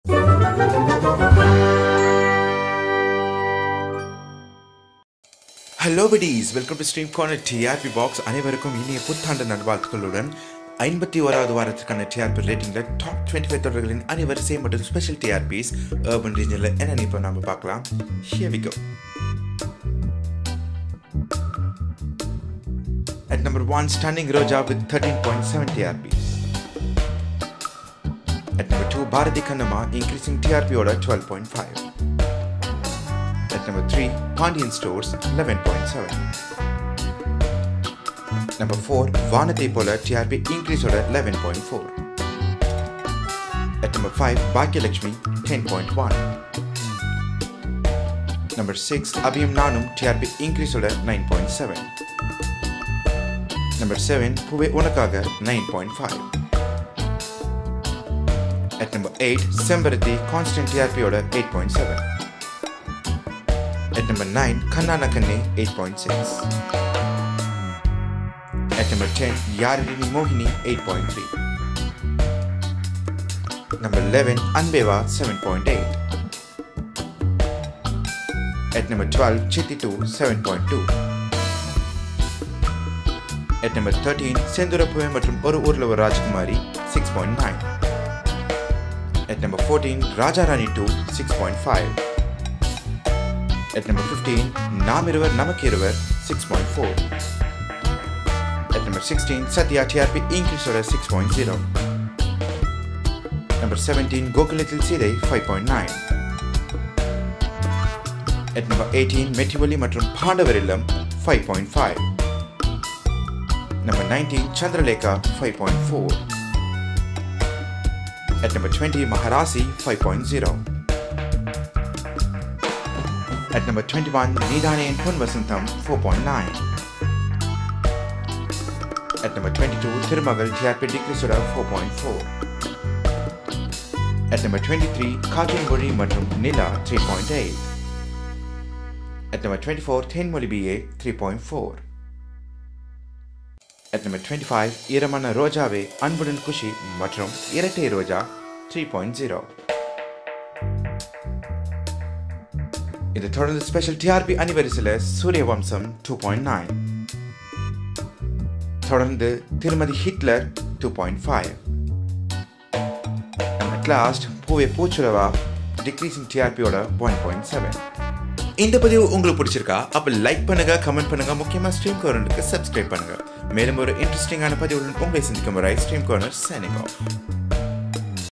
Week 51 TRP Tamil | Top 25 Serials தமிழ் | Satya | Roja | 2020 | Stream Corner
Watch Week 51 TRP Rating of Tamil television shows..